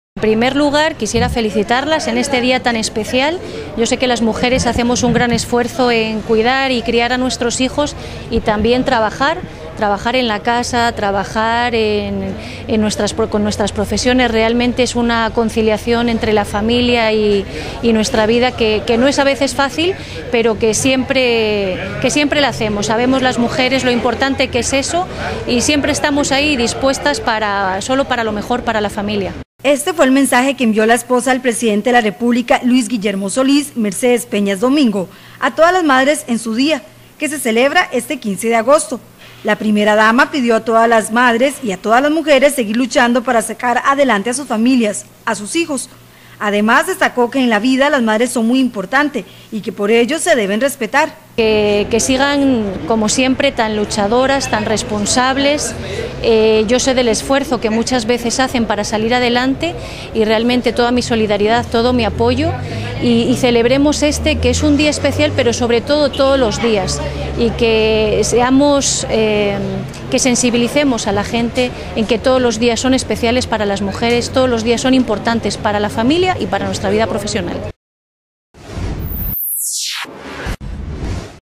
Primera Dama pide a las madres en su día seguir luchando
Este fue el mensaje que envió la esposa del Presidente de la República, Luis Guillermo Solís, Mercedes Peñas Domingo, a todas las madres en su Día, que se celebra este 15 de agosto.